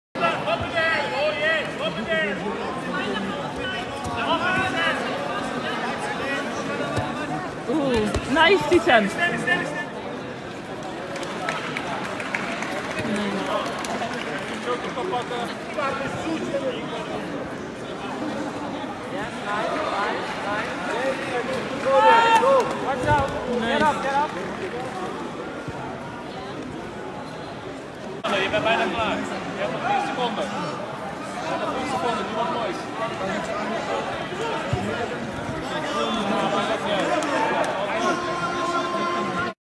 Some sweeps and escapes that work very well in BJJ competitions
As a 63~64kg guy, I'm usually smaller and lighter than my opponents in BJJ competitions in the -70kg or -155lbs division. Although I lost all those matches by points (two years ago when I just got my blue belt, many were 0:2 or by decisions), I was satisfied that I had made some nice sweeps or escapes.